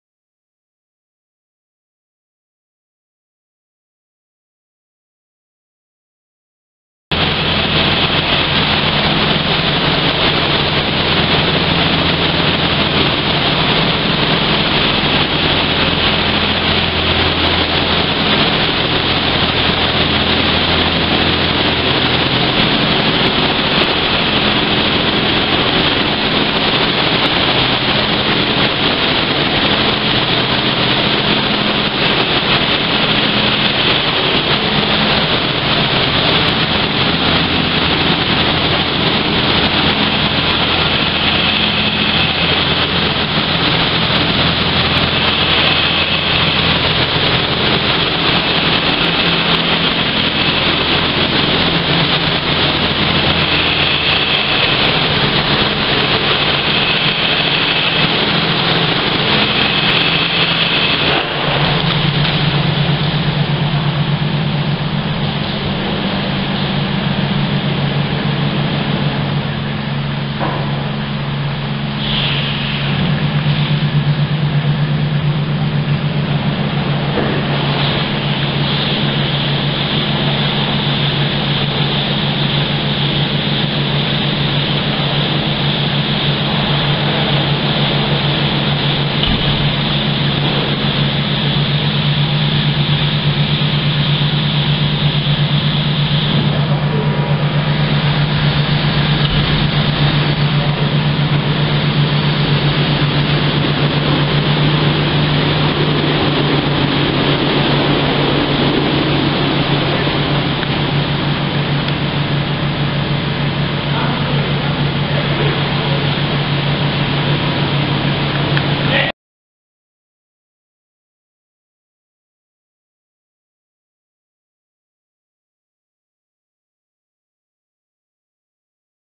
HAUTEK højtryks/børste lastbilvaskeanlæg hos Dantra A/S
Det nye HAUTEK lastbil vaskeanlæg er kommet til Danmark, leveret og monteret hos transportfirmaet Dantra A/S i Hjøllund
Det første anlæg fra Hautek´s leverandør ISTOBAL
Speciel højtryks system til effektivt forvask af silotankbiler og andre køretøjer.,